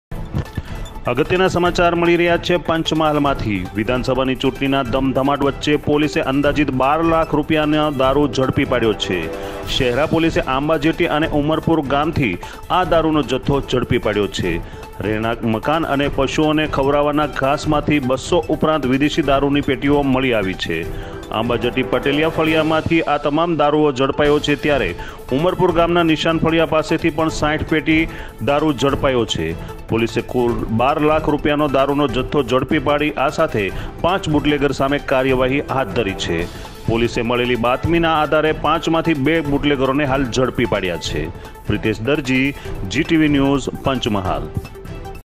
Breaking પંચમહાલ :- ચૂંટણીના ધમધમાટ વચ્ચે પોલીસે અંદાજિત 12 લાખ રૂપિયા નો દારૂ ઝડપી પાડ્યો...|| GTV
Breaking પંચમહાલ :- ચૂંટણીના ધમધમાટ વચ્ચે પોલીસે અંદાજિત 12 લાખ રૂપિયા નો દારૂ ઝડપી પાડ્યો...|| GTV_GUJARAT_NEWS